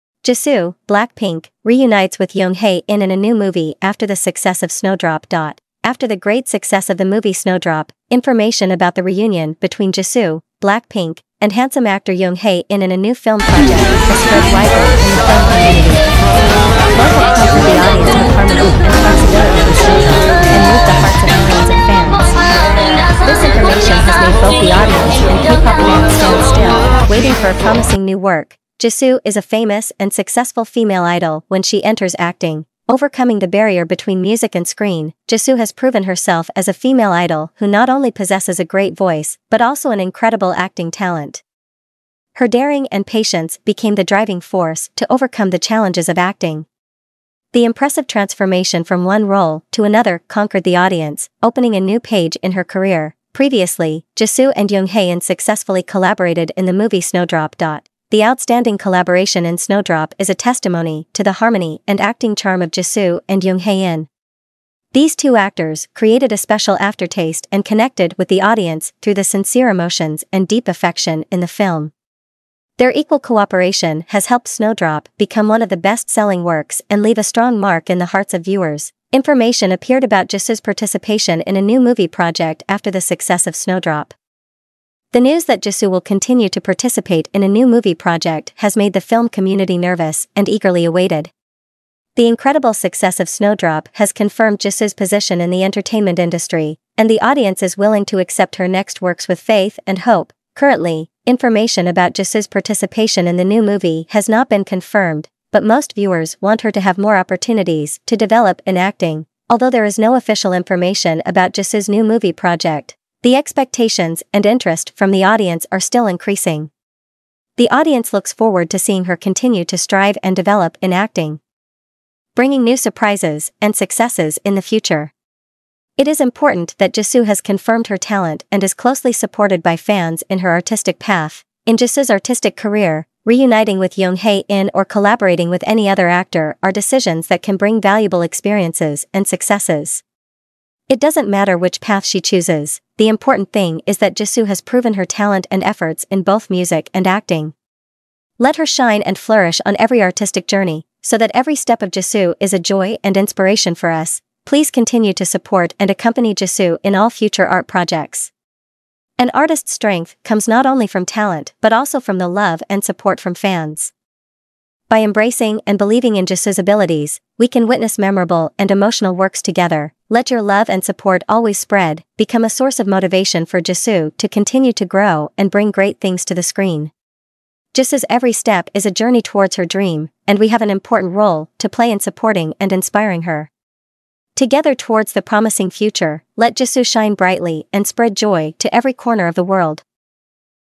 Jisoo (BLACKPINK) and Jung Hae In reunite in a new movie after the "Snowdrop" fever! - ACN News
Jisoo (BLACKPINK) and Jung Hae In reunite in a new movie after the "Snowdrop" fever!  Both have conquered the audience with harmony and flexibility in Snowdrop, and moved the hearts of millions of fans. This information has made both the audience and K-pop fans stand still, waiting for a promising new work.